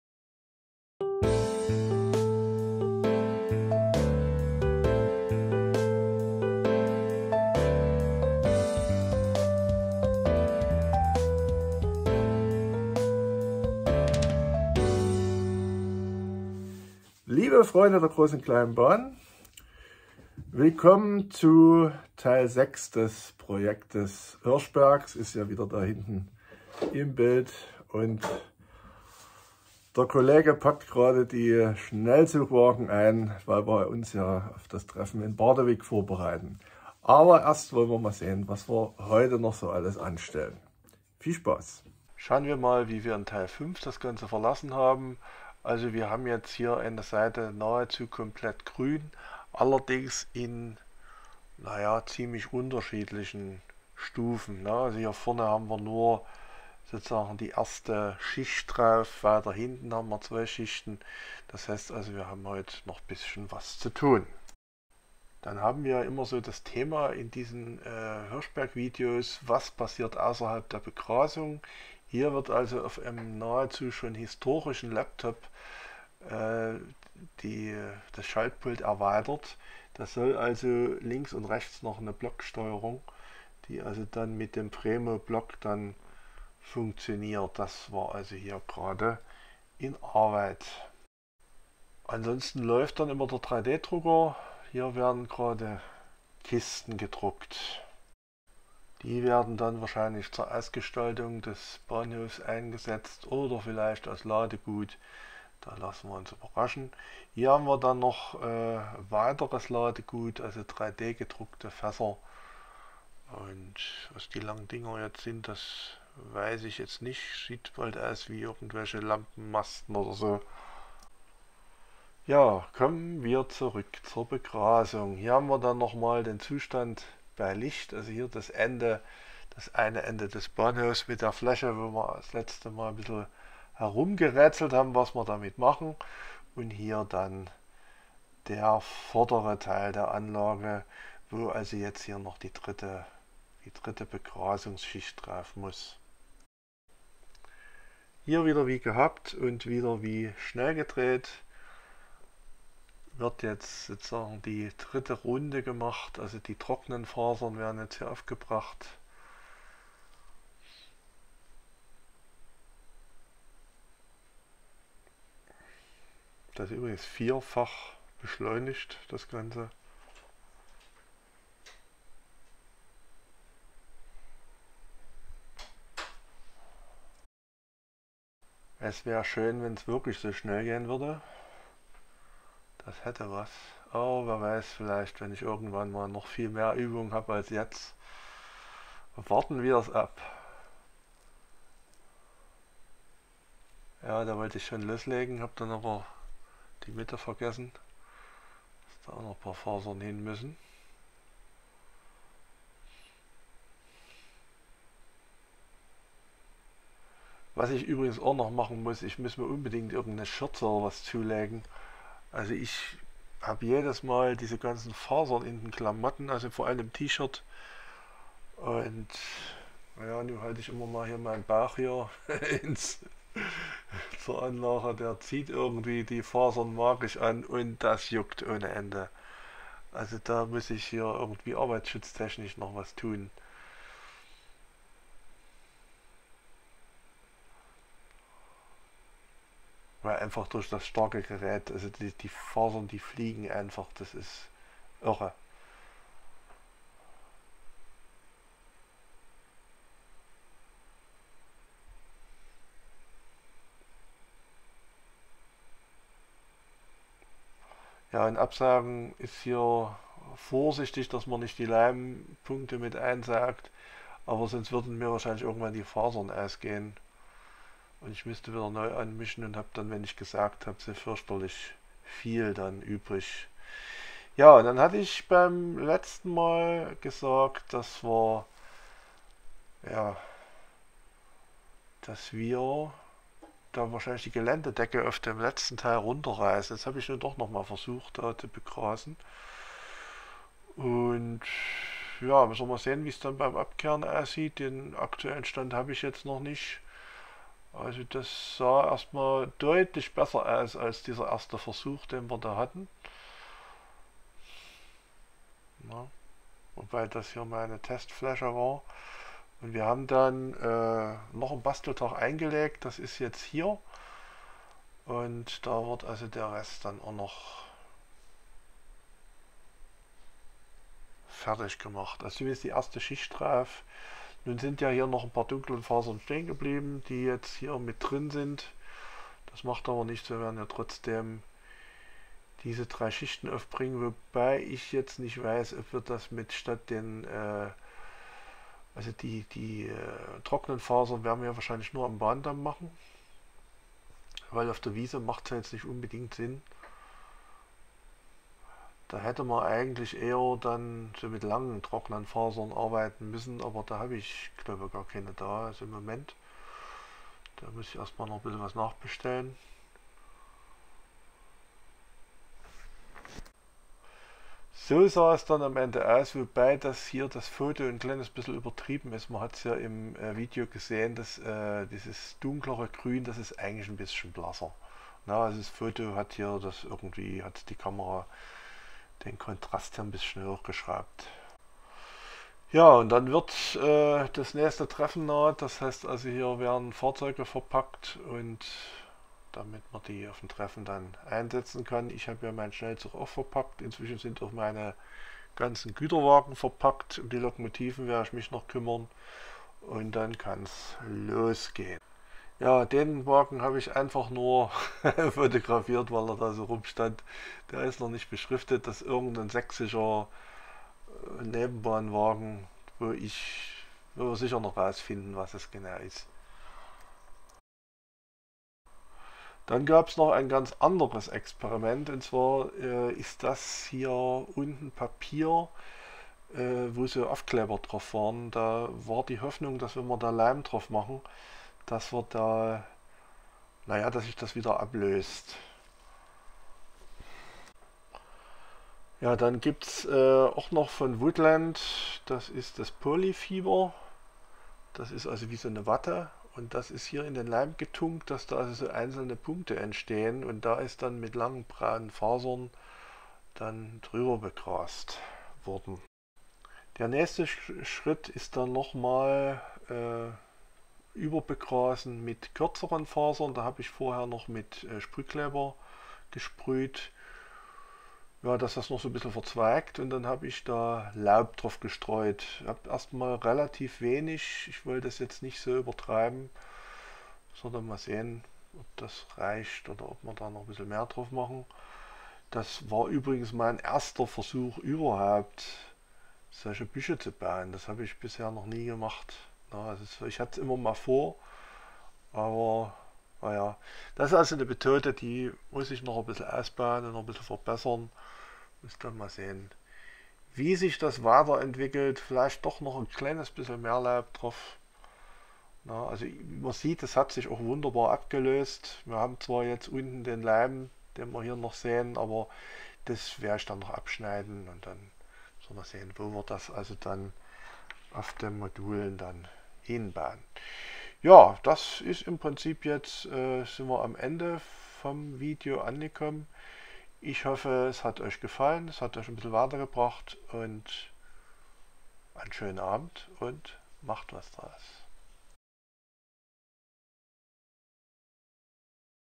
Hirschberg Teil 6 (149)
Schaltpult, 3D Druck, der aktuelle Stand beim Begrasen, Vorbereitungen auf das Fremotreffen in Bardowick und der Versuch Büsche zu bauen.